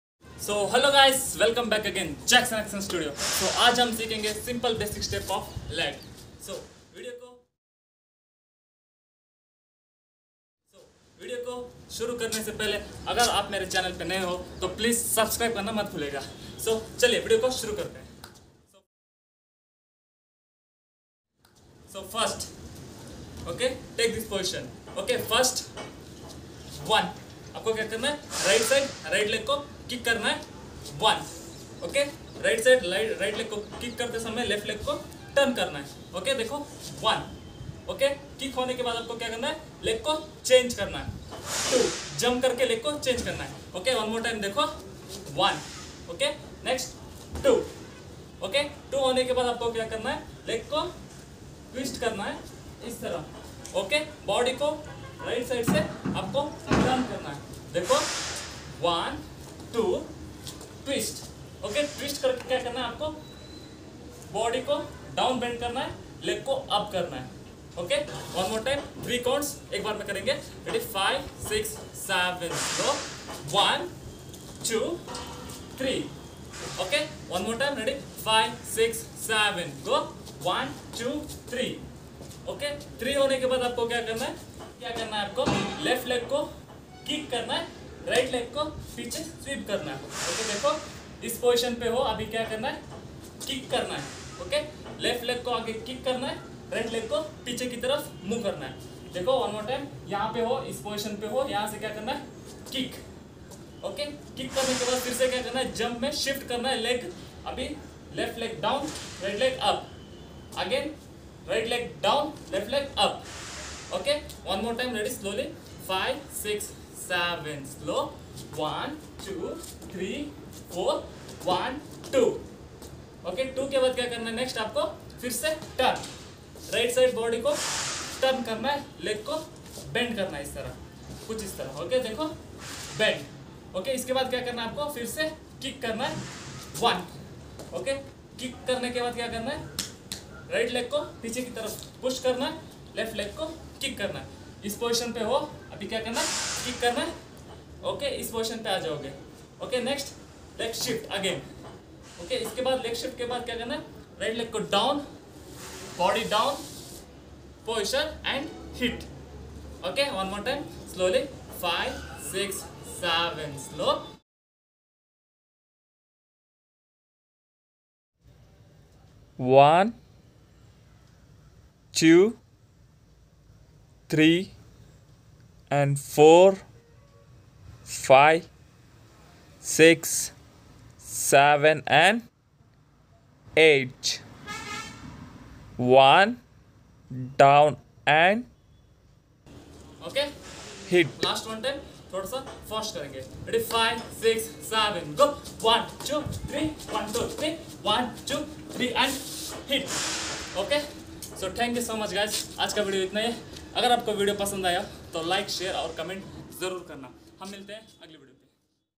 Simple basic step of leg | Footwork tutorial | basic dance move Hip-Hop
Class - 08
Simple basic step of leg  Footwork tutorial  basic dance  move for Hip-Hop  By Jackson Action Studio
Do watch learn share and create

Personal ac:- @dsbharathdhatt | @dsbharathdhatt

Photo and videography ac;- @bphotographyhub

Jackson Action Studio